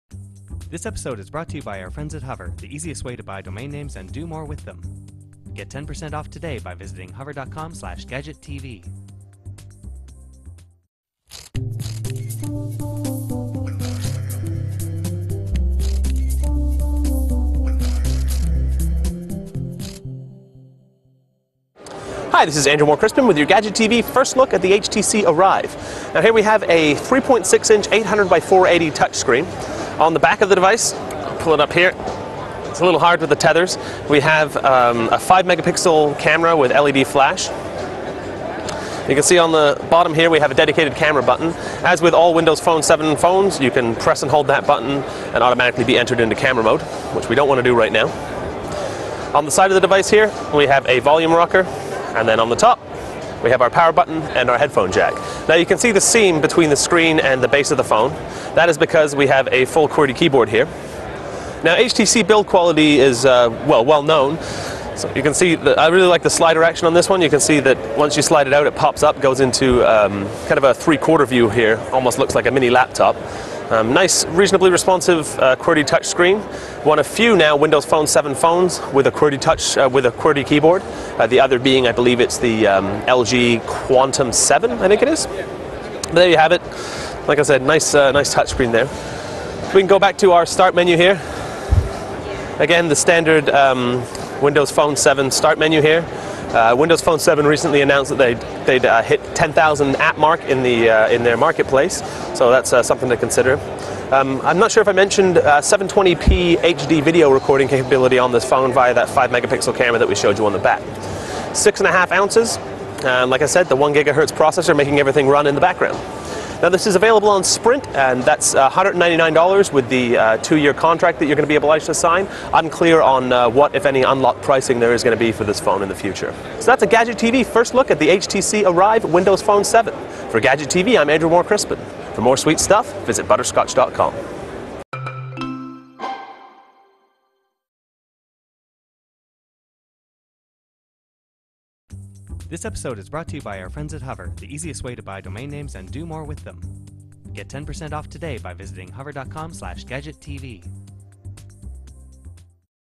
First Look - HTC Arrive on Sprint at CTIA 2011
HTC is widely recognized as making some of the best smartphone hardware going. The HTC is the Arrive is the second WP7 phone to feature a full QWERTY keyboard. While WP7 is playing catch-up a little, phones like this do nothing if not help the stock of HTC @ butterscotch.com